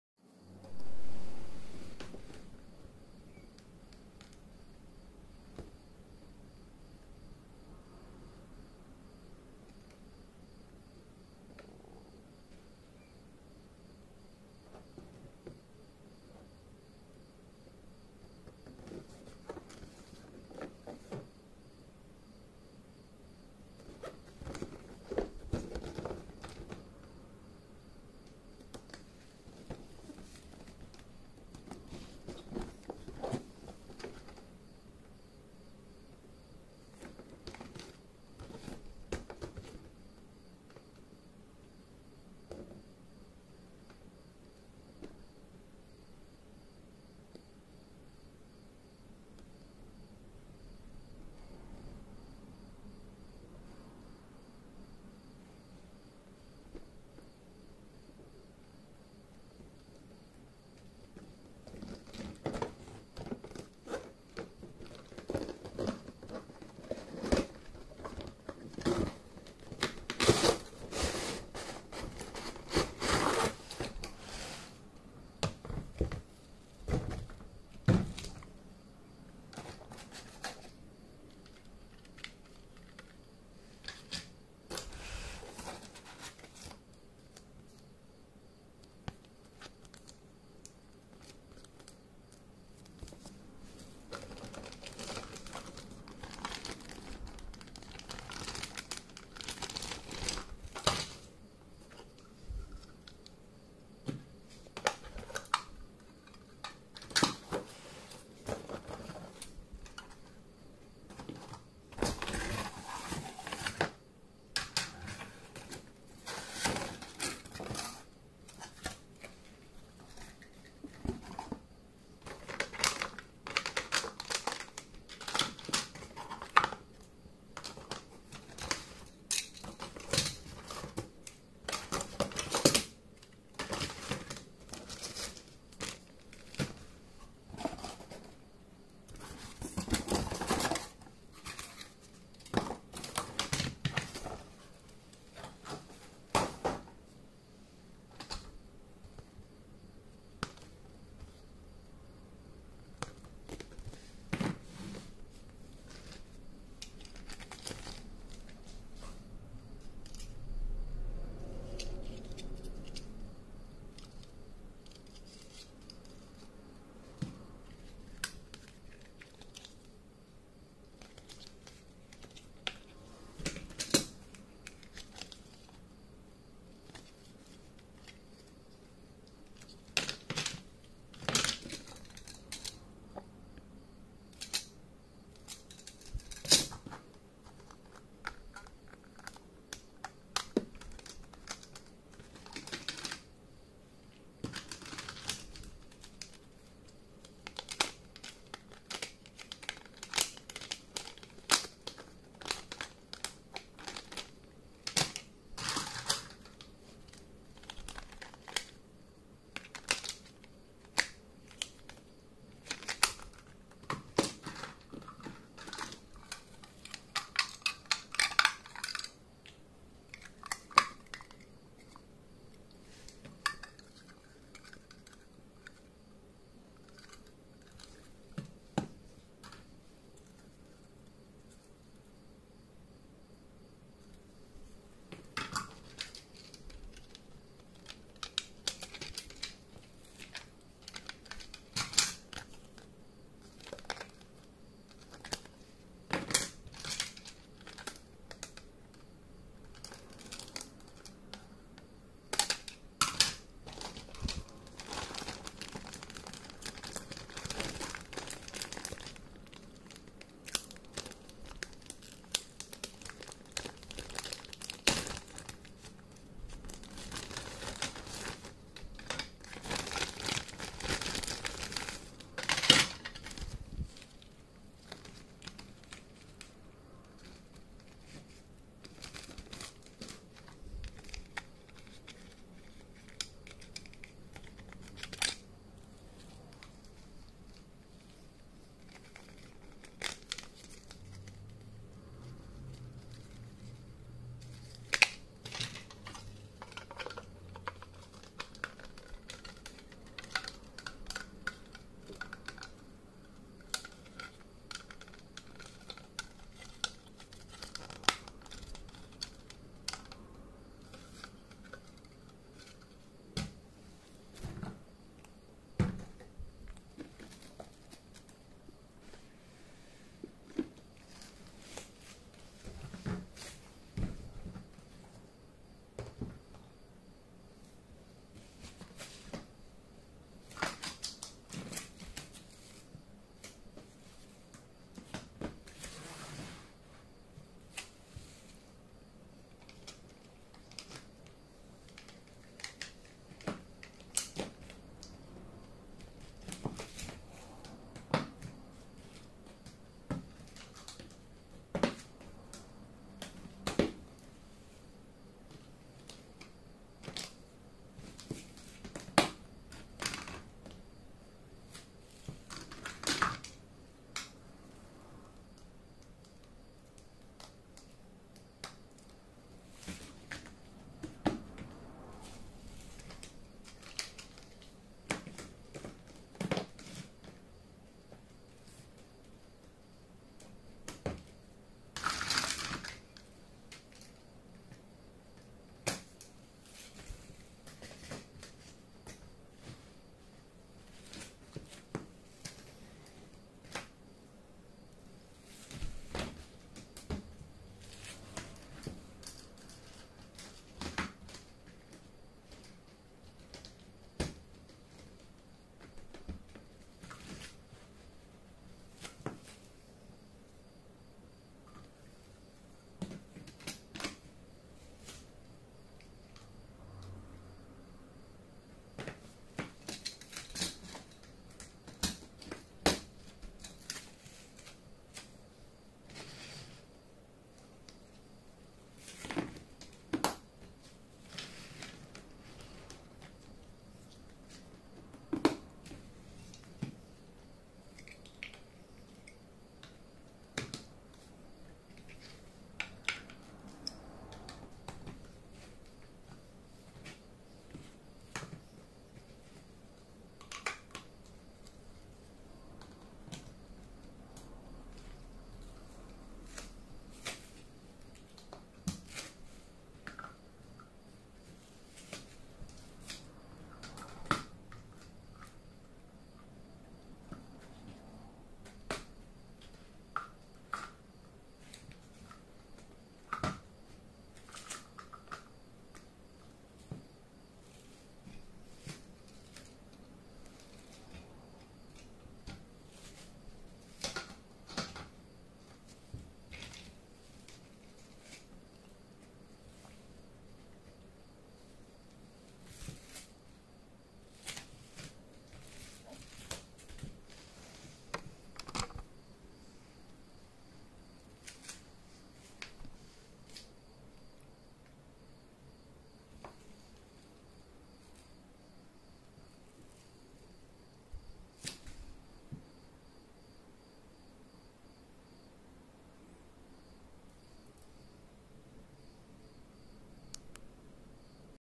プラレール景色のカラーレールキット〜花と鉄道〜の開封＆組み立て動画。
我が家のプラレール紹介。

今日は、景色カラーレールキット花と鉄道のプラレールの紹介です。

こちらのレールセットは、今年の5月にアマゾンで購入したプラレールです。

景色カラーレールセットは、春、夏、秋、冬の季節に応じて発売されている、カラーレールセットです。

先程は、第1段となる、雪と鉄道のレールキットを紹介しましたが、今日紹介するのは、第2段となる花と鉄道です。

第2段となる、景色カラーレールキットの花と鉄道は、桜の色をモチーフにしたレールセットとなっており、レールのカラーは、おしゃれなピンク色カラー、ニュー坂レールは、紫色カラーになっています。

このセットだけで、約3種類のレイアウトを作る事が出来ます。

景色カラーレールキットは、後海と鉄道、紅葉と鉄道の2種類がありますので、こちらの2種類のレールキットも、後ほど紹介する予定です。